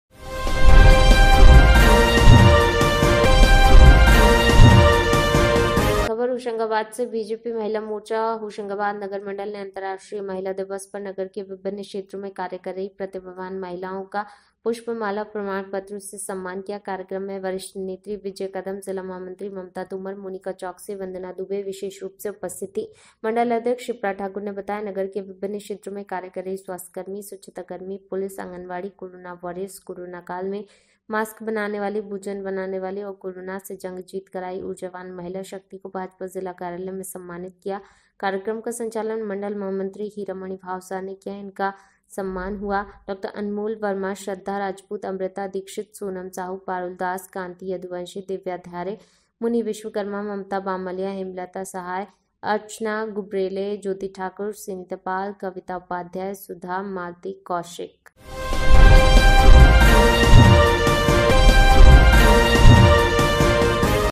भाजपा महिला मोर्चा ने किया नगर की प्रतिभावान महिलाओं का सम्मान
दिव्या घ्यारे को प्रशस्ति पत्र देकर किया सम्मानित

होशंगाबाद। भारतीय जनता पार्टी महिला मोर्चा होशंगाबाद नगर मण्डल ने अंतराष्ट्रीय महिला दिवस पर नगर के विभिन्न क्षेत्रों में कार्य कर रही प्रतिभावान महिलाओं का पुष्पमाला एवं प्रमाण पत्र से सम्मान किया।